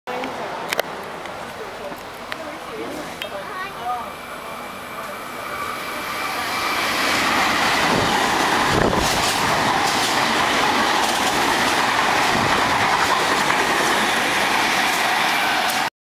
V set passing Mt Druitt
City Bound train passing Platform 2 at Mount Druitt.

None of my videos should be put on other websites that aren't necessary.